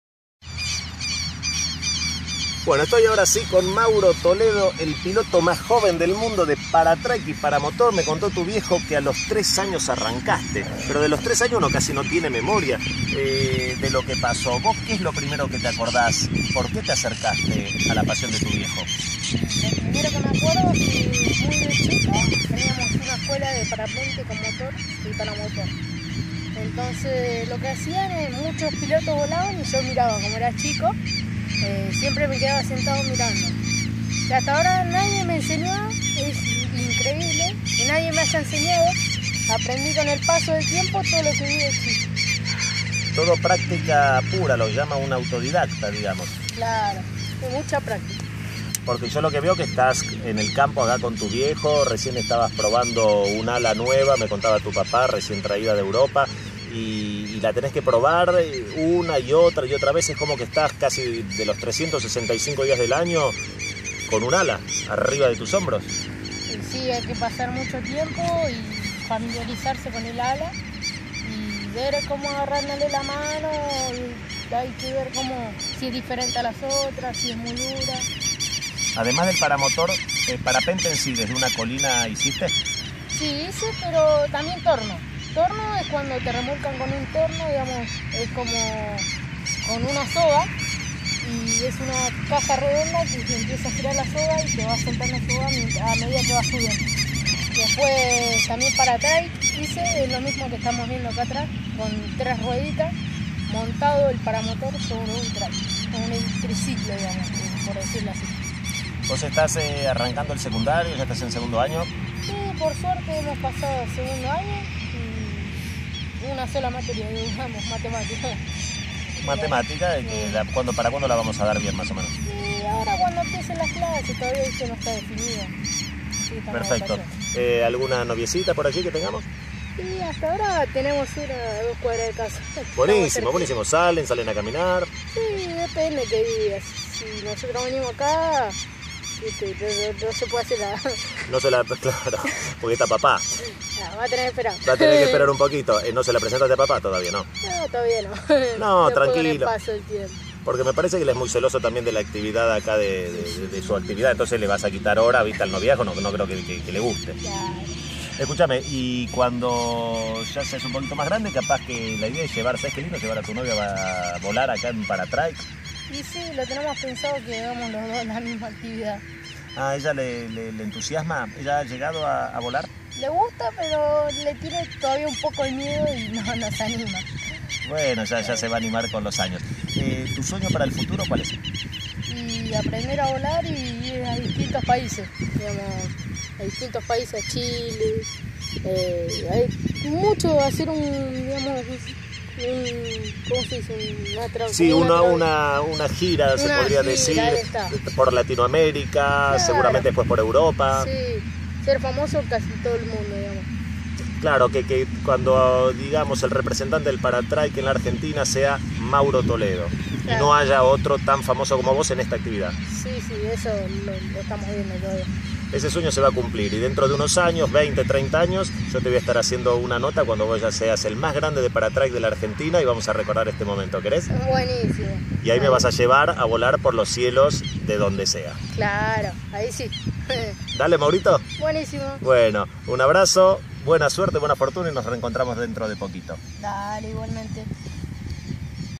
Entrevista a Mauro Toledo , el piloto mas joven de paratike
Mauro Toledo con tan solo 13 años , es el piloto mas joven del mundo . Empezó su pasión a los 3 años de edad y voló por primera vez a los 7 años.

Un chico que encontró el amor de volar y pone una pasión y unas ganas terribles.

ES DIVERTIDO , SIMPÁTICO , TE INVITO A CONOCERLO ..

paramotorlabusqueda

Autpista ezeiza cañuelas km 61 a metros de la rotonda de cañuelas..